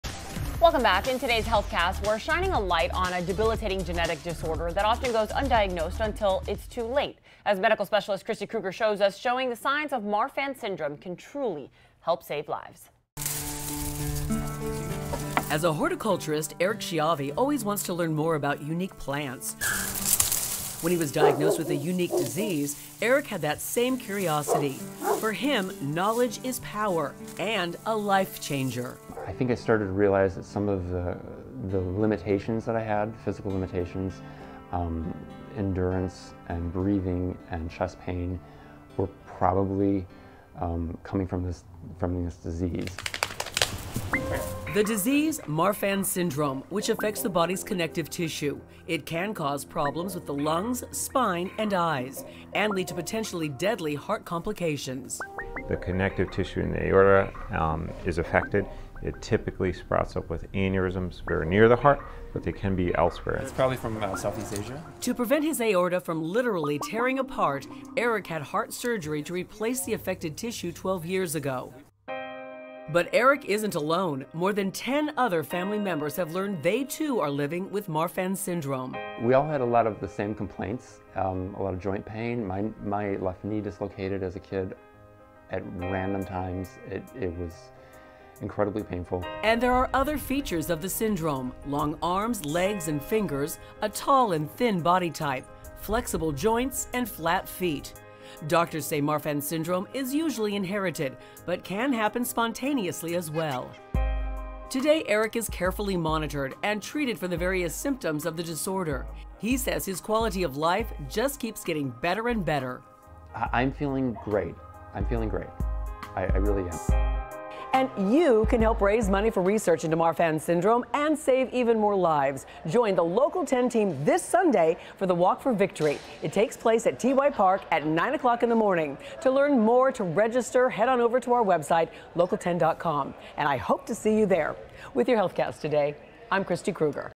What are the signs and symptoms of Marfan syndrome?
In today's Healthcast, we are shining a light on a debilitating genetic disorder that often goes undiagnosed until it's too late. As medical specialist Kristi Krueger shows us, knowing the signs of Marfan syndrome can truly help save lives.